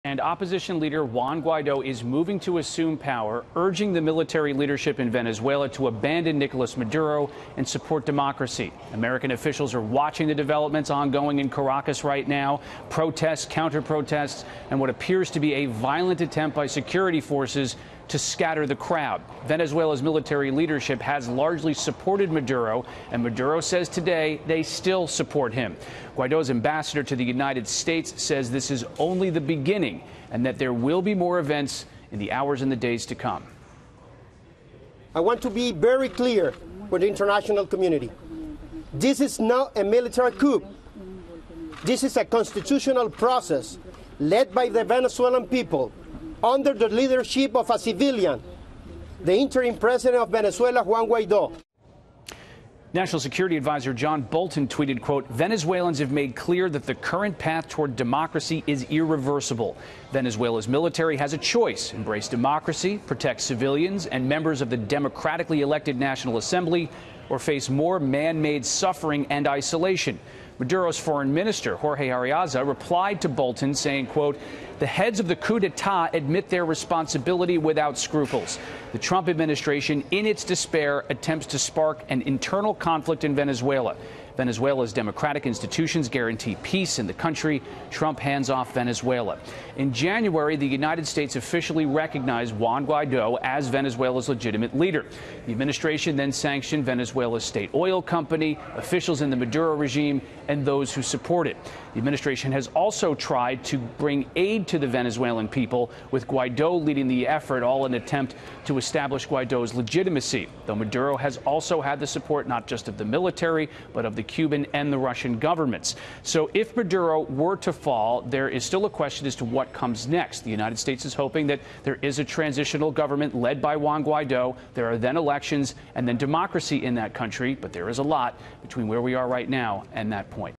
Chaos erupts in Venezuela amid military uprising
Opposition leader Juan Guaido moves to assume power, urging Venezuela's military leadership to abandon Nicolas Maduro and support democracy; Rich Edson reports from the State Department.